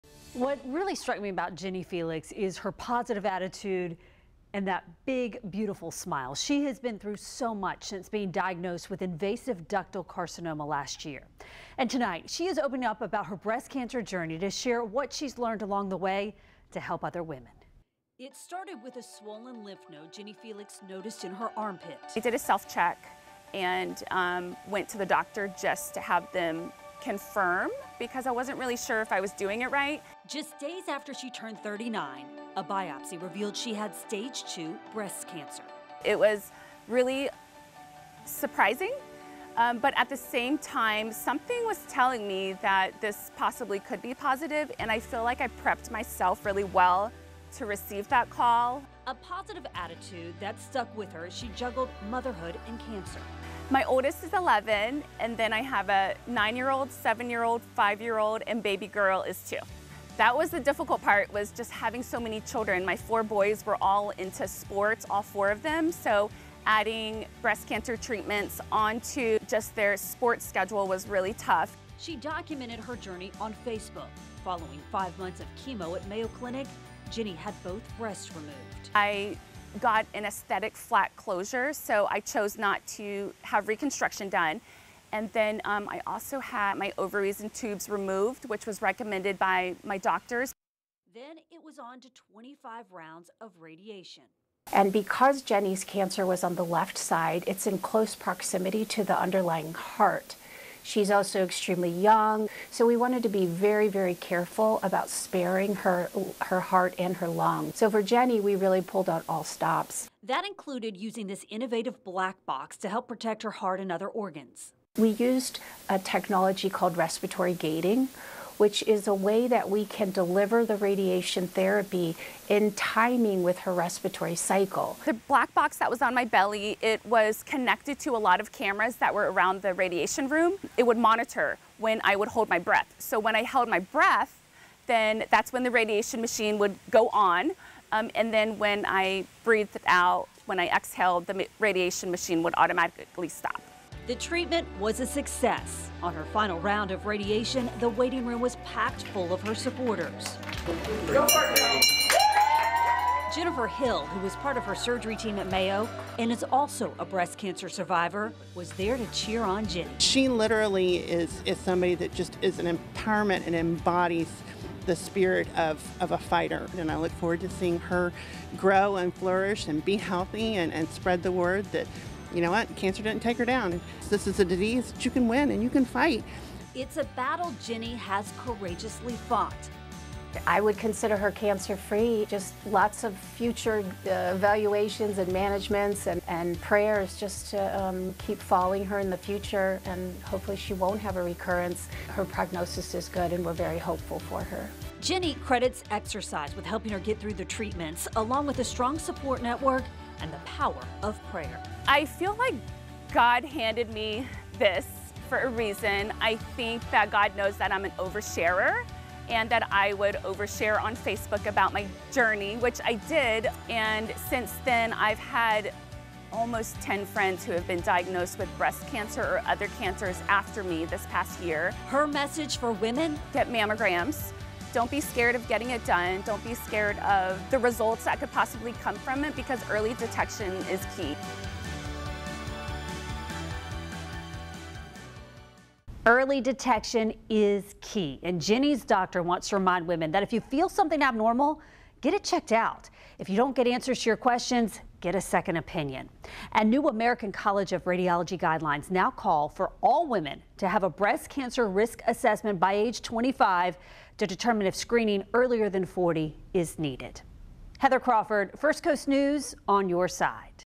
St. Johns County mother opens up about breast cancer journey: “I feel like God handed me this for a
Jenny Felix was diagnosed with breast cancer in August 2022. One year later, she is sharing her journey to help remind women early detection saves lives.